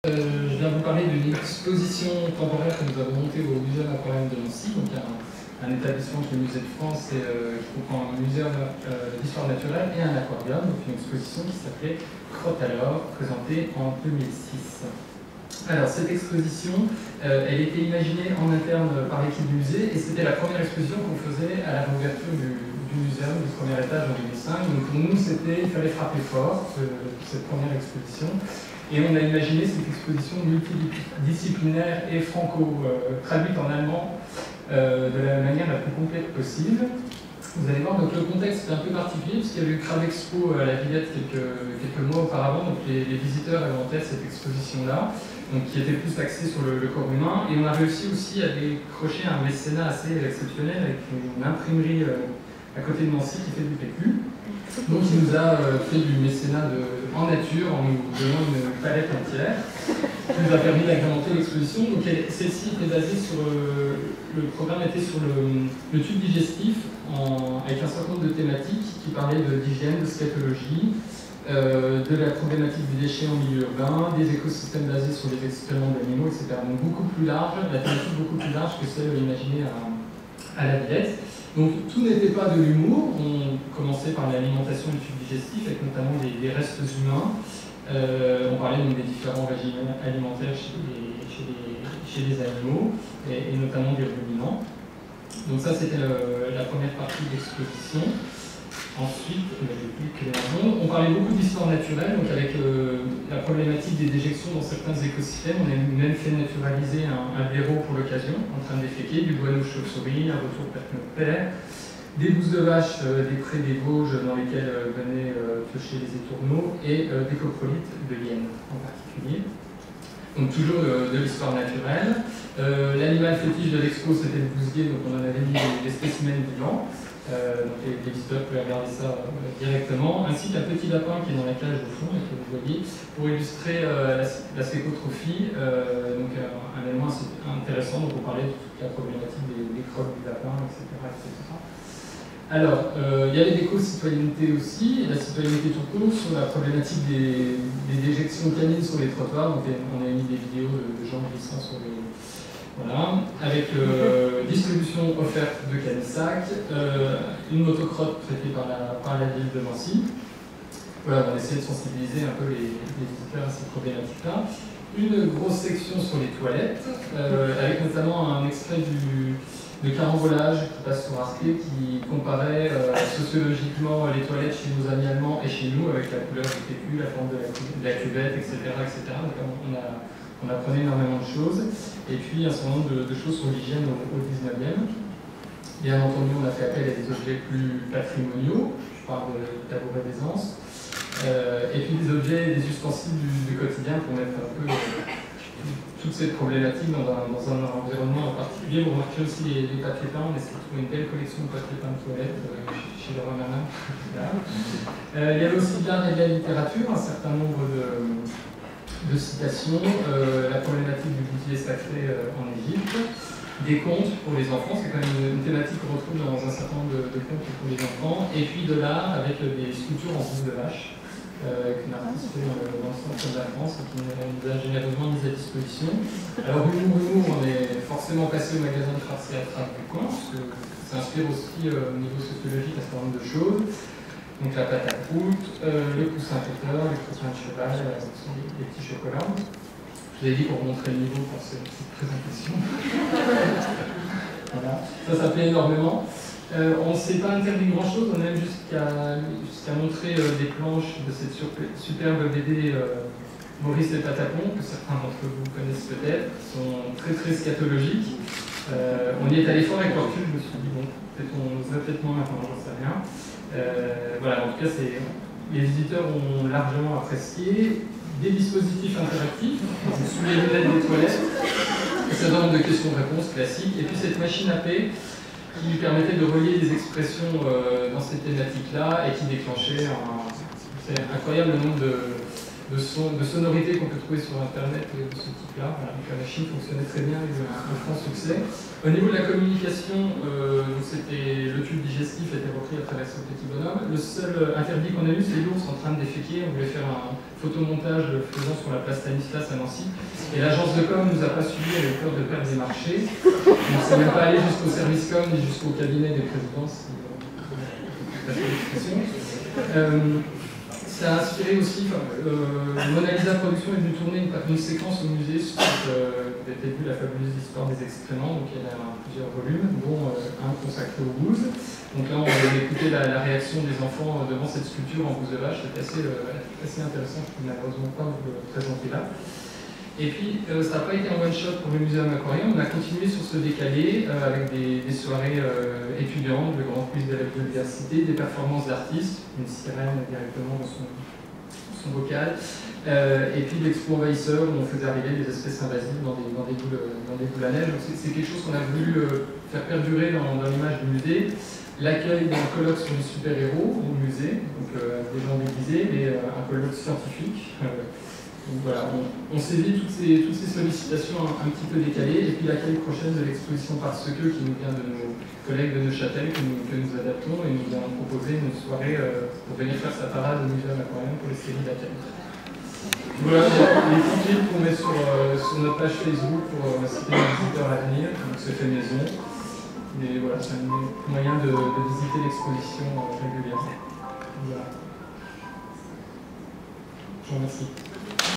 Congrès amcsti 2012 "Crotte alors ! " - Pierre-Antoine Gérard
Présentation de l'exposition « Crotte alors ! » du Muséum Aquarium de Nancy au 30eme congrès de l' amcsti  à Toulouse.